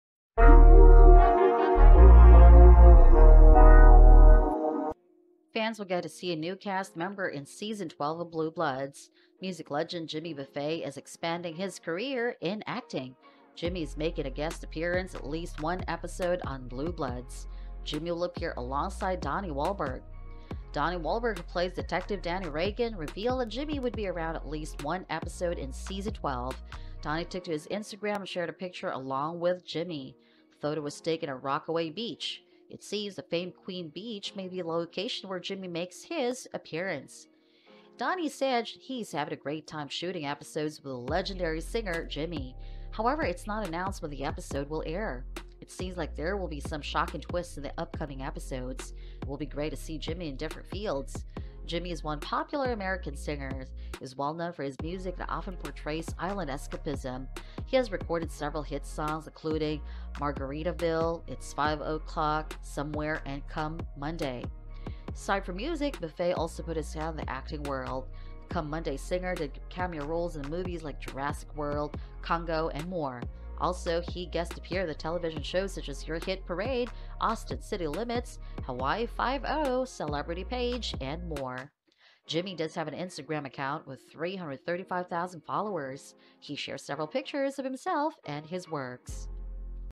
Blue Bloods Season 12 Spoilers: Jimmy Buffet's Guest Appearance on New Episode
A music legend Jimmy Buffett makes a guest appearance in an upcoming episode of season 12, Blue Bloods.